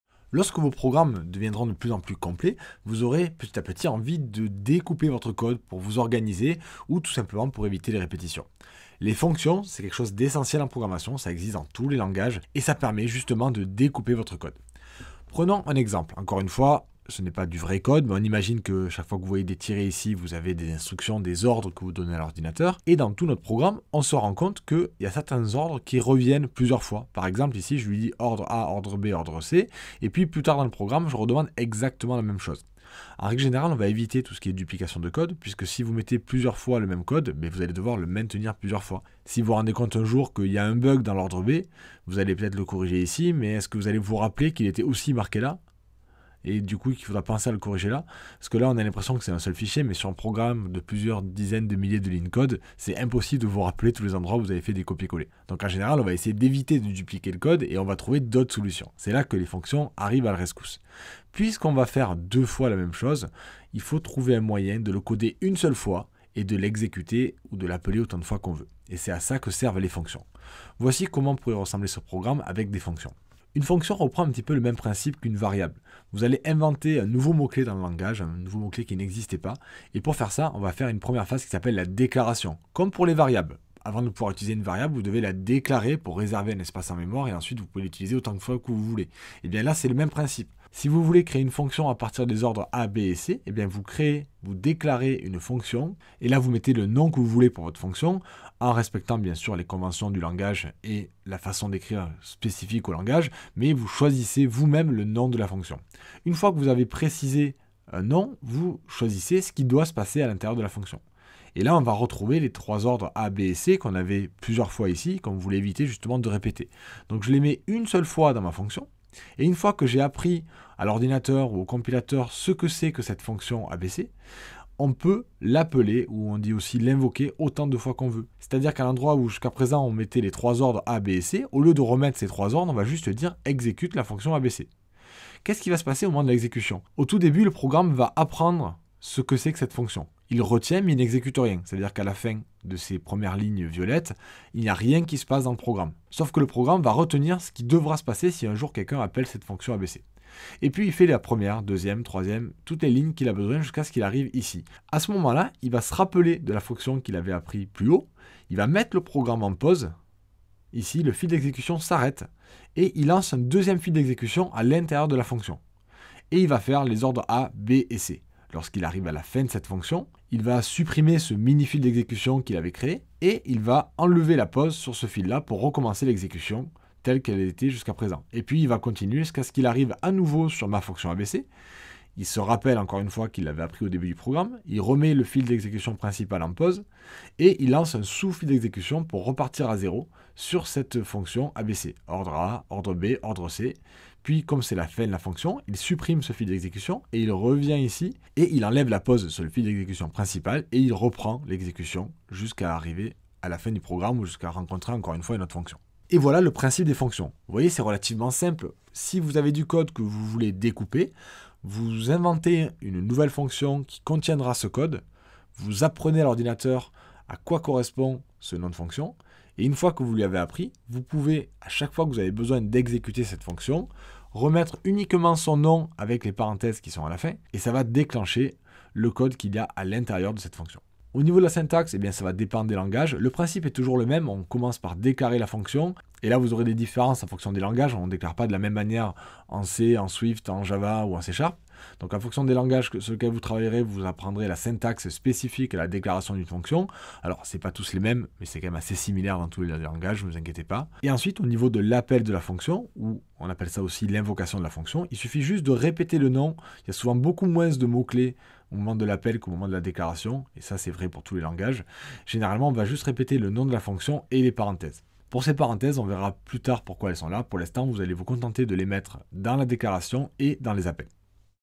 Apprendre à programmer : Les fonctions (extrait du cours complet)
Comprenez rapidement le concept des fonctions en programmation.
Cette vidéo provient du cours complet "Apprendre à programmer (en Swift)"

Devenir programmeur est l'un des meilleurs choix possibles à l'heure actuelle et je ferai tout mon possible pour vous y aider 👍
Happy coding!
Maxime